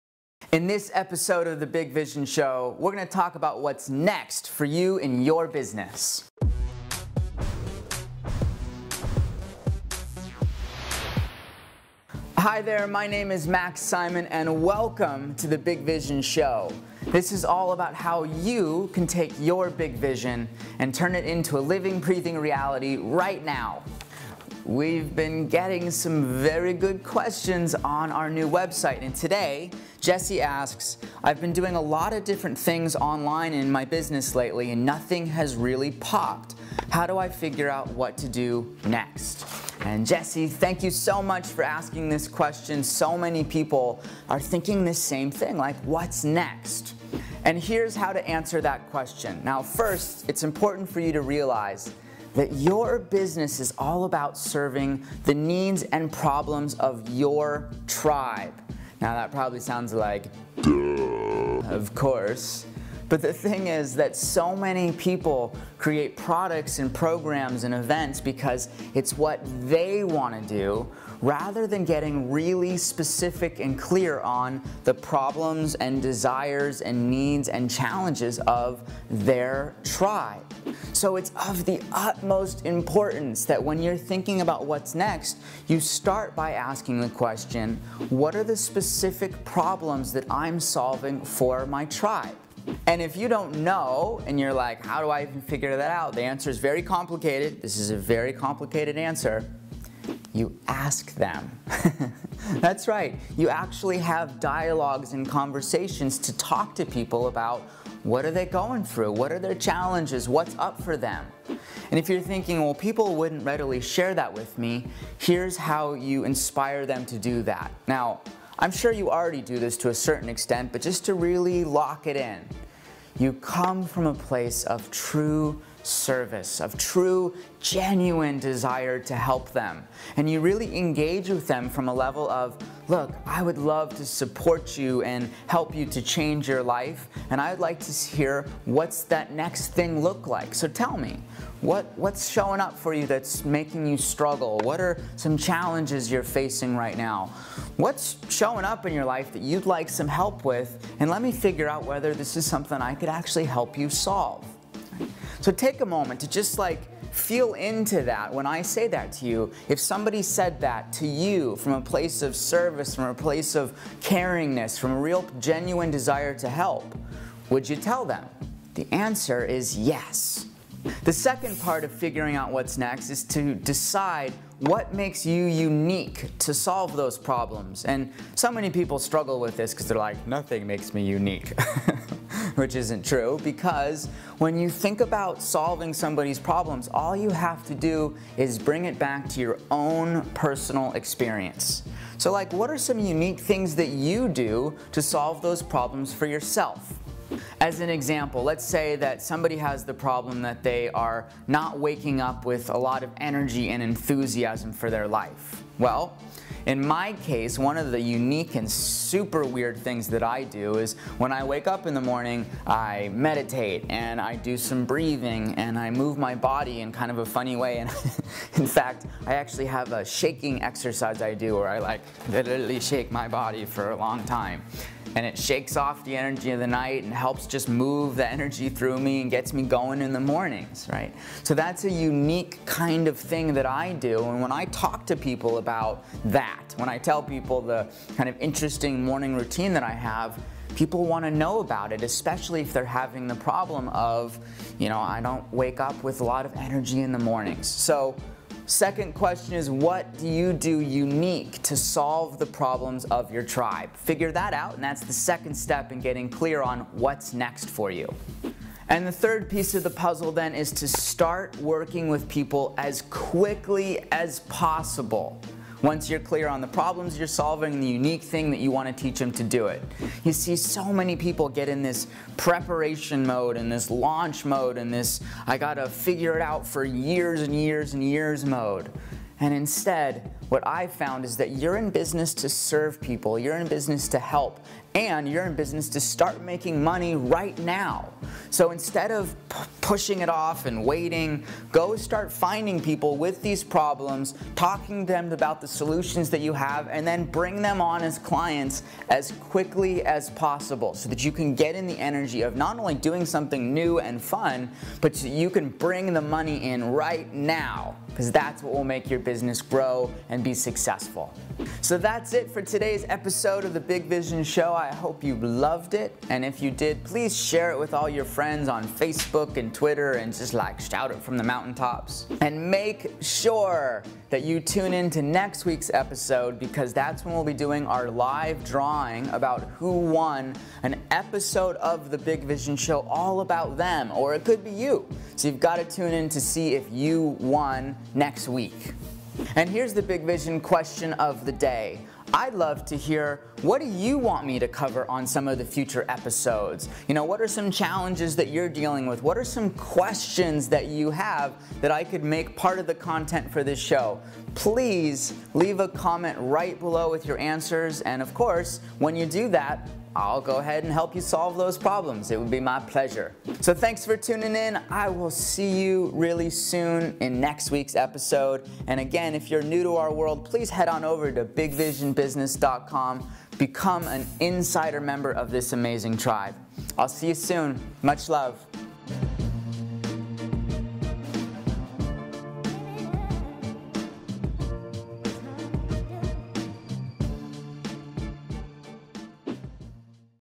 What's Next For Your Business? - Discover what is next for your business
What is next for your business? Max Simon shares the 3 most important steps to discover what you should do NEXT in your Big Vision Business so that you can start truly helping more people and start bringing in more money RIGHT NOW!